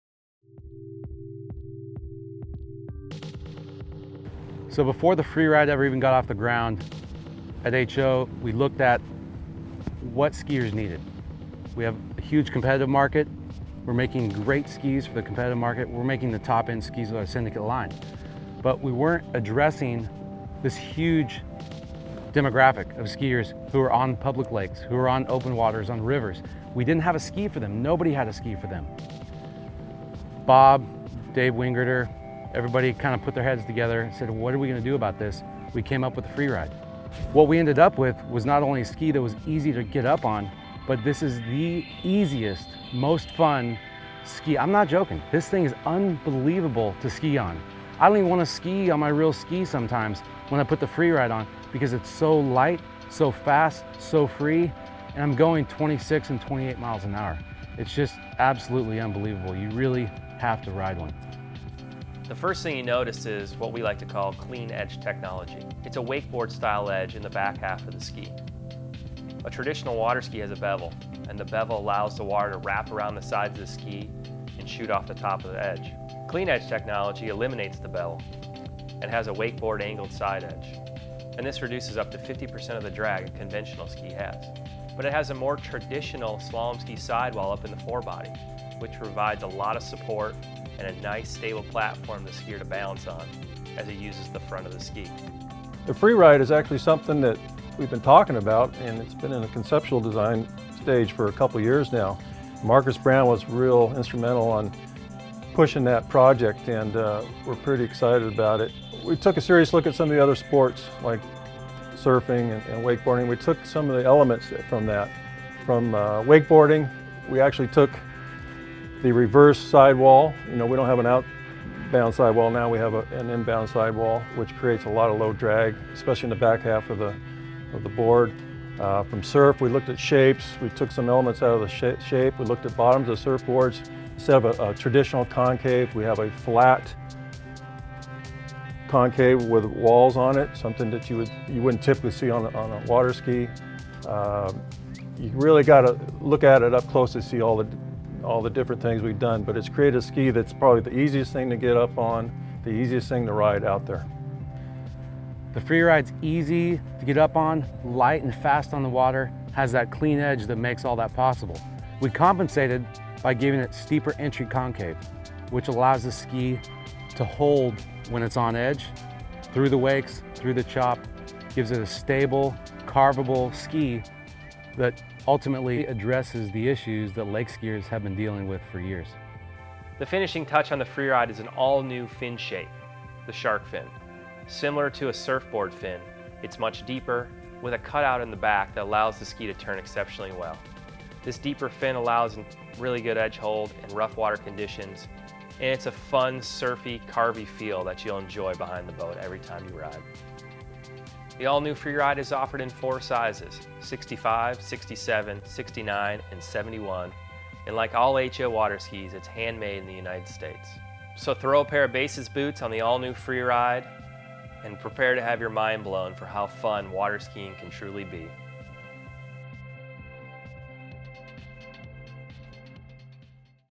2013 HO Freeride
The Freeride is not only a new take on water ski design, but a new take on waterskiing all together. The days of slalom skiing being constrained to private locations, pristine conditions, and behind select watercrafts is over. This ski is to be rode virtually anywhere, anytime, behind anything, and in any ski style you can come up with. The Clean Edge Technology allows the water to release "cleanly" from the bottom surface of the ski without wrapping up around the sidewall of the ski. This "clean" release of the water reduces ski drag up to 50% over conventional ski design allowing the skier to more easily glide across the water and pop up easy from deep-water starts. The Freeride's Carve Rails allow for amazing edge hold when laying down powerful arcs and turns. The Carve Flex allows active rocker carving and teamed with the surf inspired Shark Fin, maximizes the ski's carvability in even the most trying water conditions. Lastly, the Hybrid Sidewall design gives you speed where you want it and support where you need it for the most fun, intense and chill ride wherever you decide to ride freely!